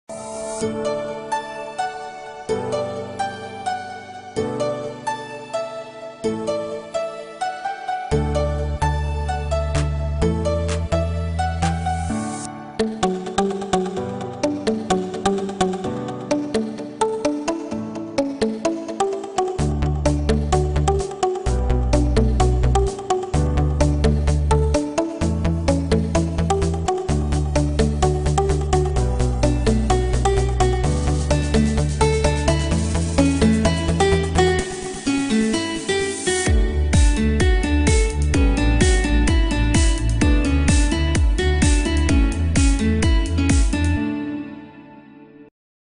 Taking Pictures Under the Water with OUKITEL WP10 5G Rugged Smartphone
Choose underwater mode and record the wonderful water world with a click.